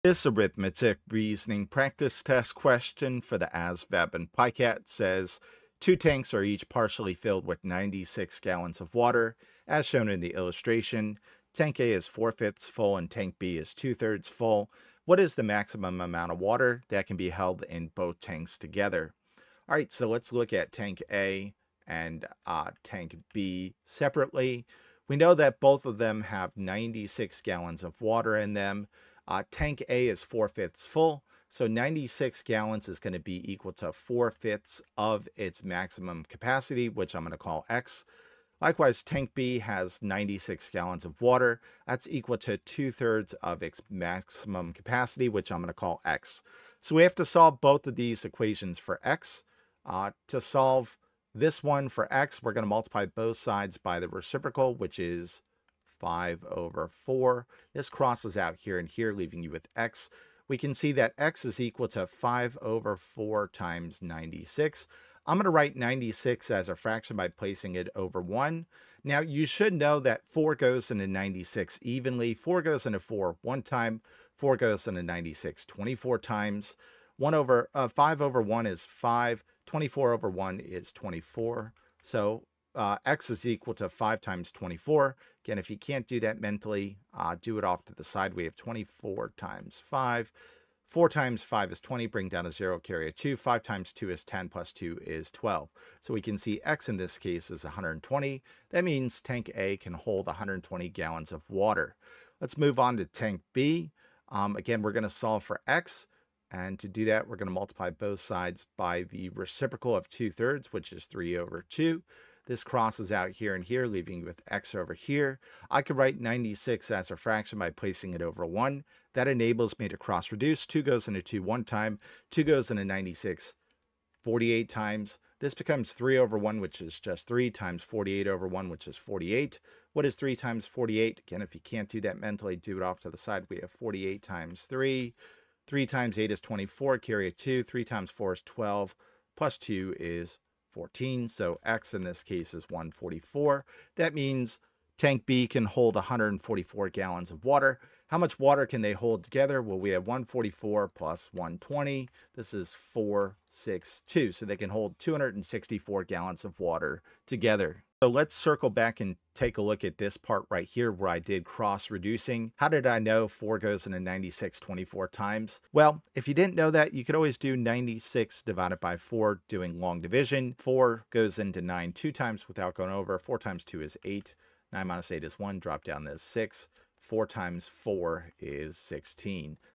ASVAB & PiCAT Arithmetic Reasoning Challenge Q: Translating Statements with Fractions
​​​​​In this video, Grammar Hero works out a practice test question that requires you to translate statements into equations and then solve equations involving fractions. This question should closely mirror what you should expect to see in the Arithmetic Reasoning (AR) subtest of the Armed Services Vocational Aptitude Battery (ASVAB) as well as the Pre-screening internet-delivered Computer Adaptive Test (PiCAT). ​​​​​grammarhero ​​ASVAB​​​​​